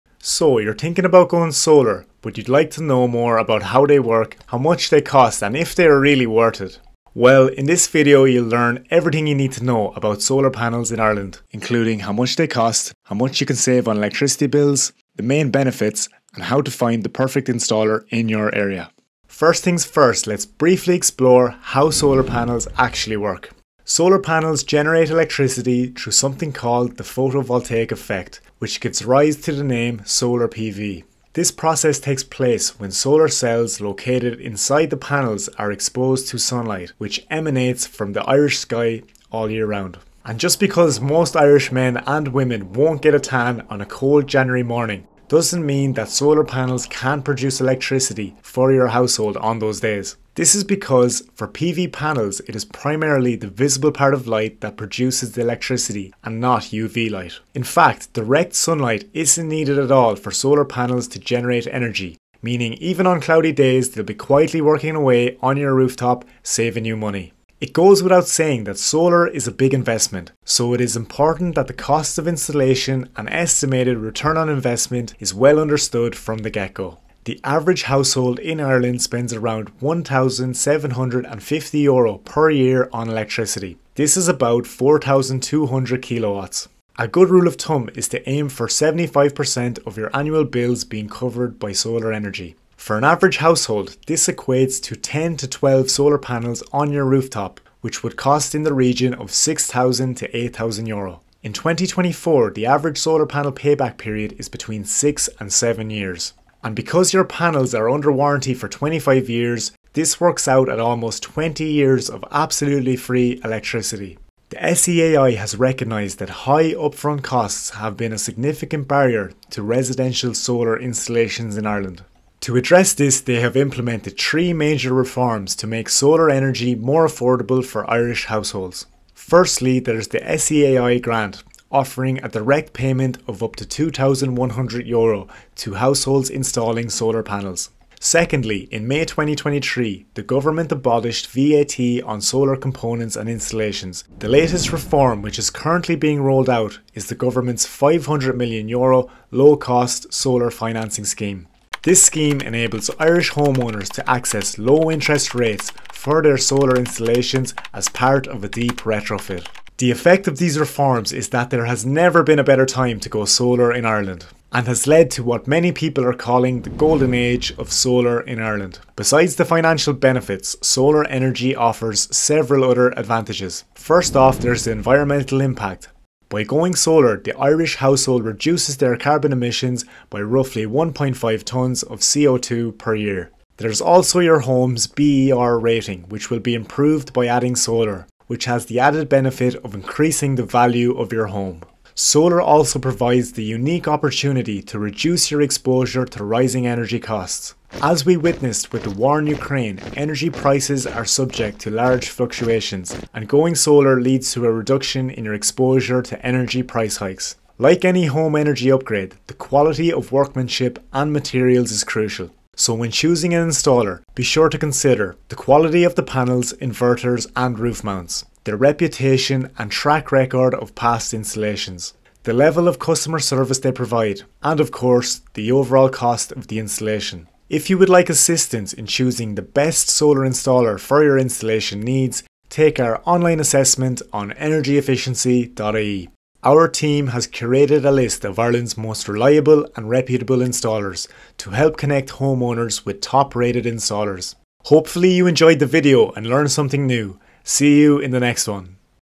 Solar Panels in Ireland - Everything you Need to Know
Solar panels are a great way of reducing your household bills - and they're great for the planet too, compared to fossil fuels. If you're looking for a reliable solar panel installer in your county, or just want to find out more about solar energy, visit EnergyEfficiency.ie today!